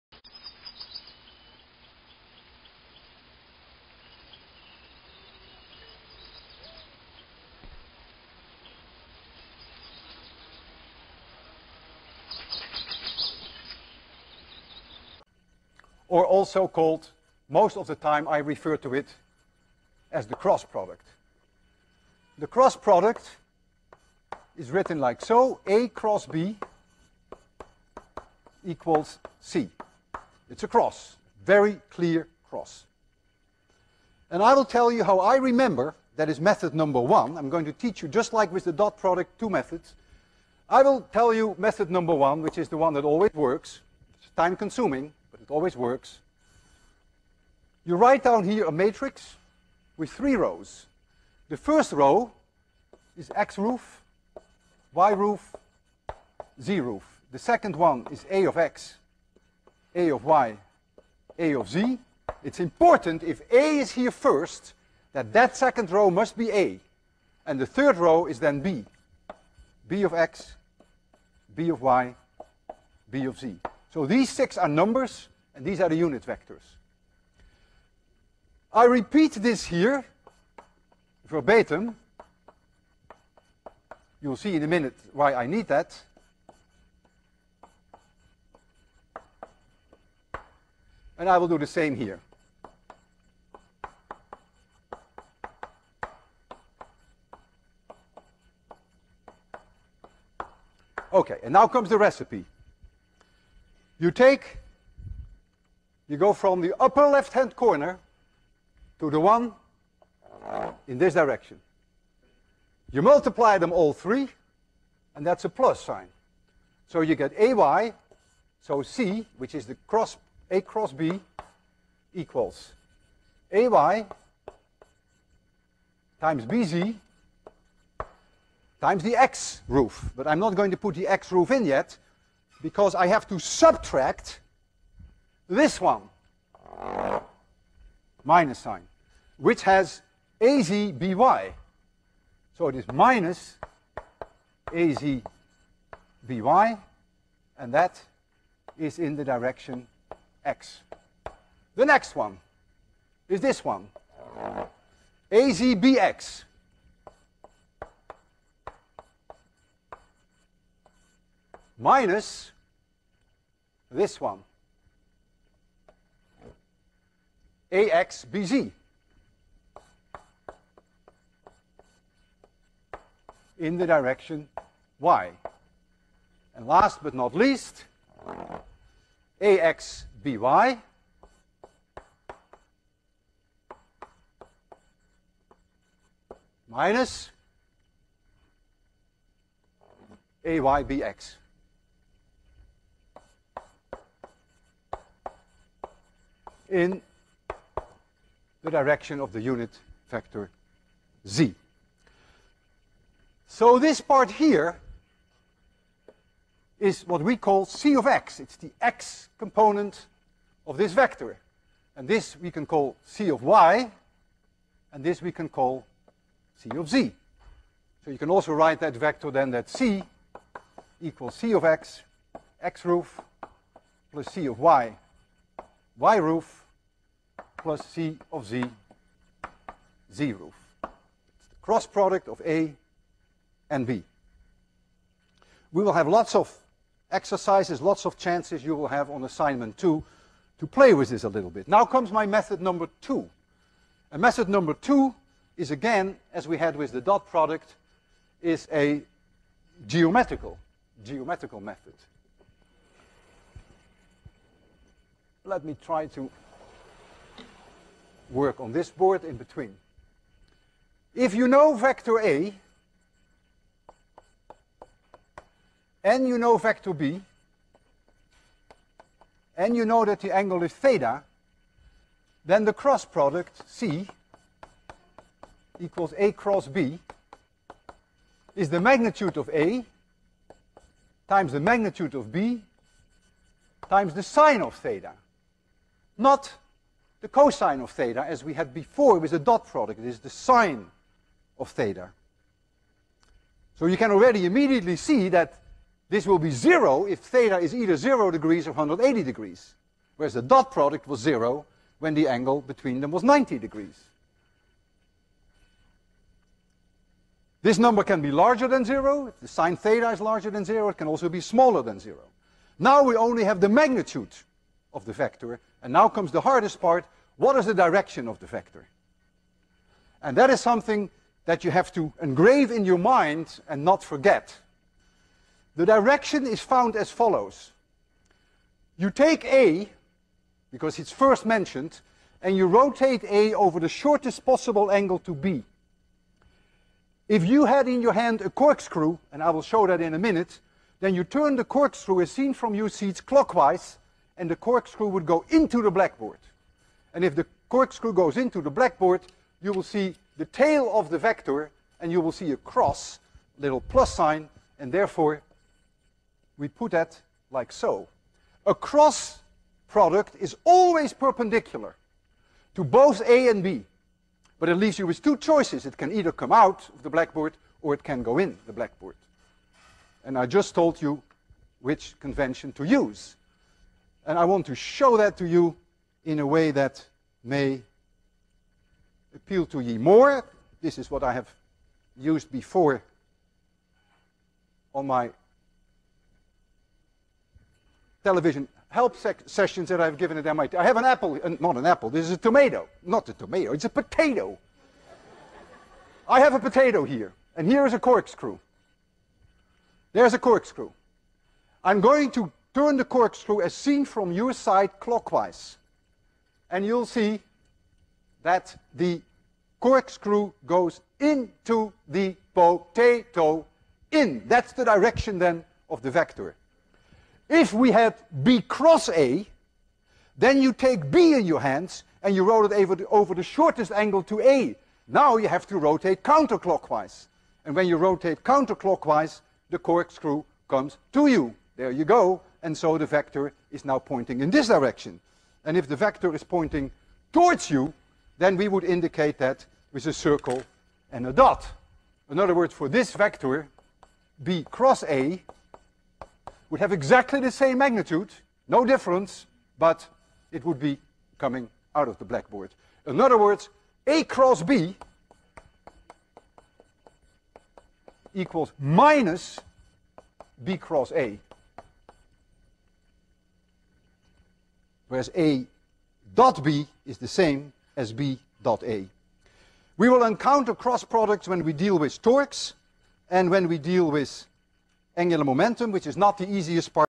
محاضرة المتجهات - الجزء الثالث
محاضرة الدكتور والتر لوين عن المتجهات ويشرح فيها مفهوم المتجهات وكذلك جمع وطرح المتجهات وضرب المتجهات وعدد من المفاهيم الأخرى ، مشاهدة ممتعة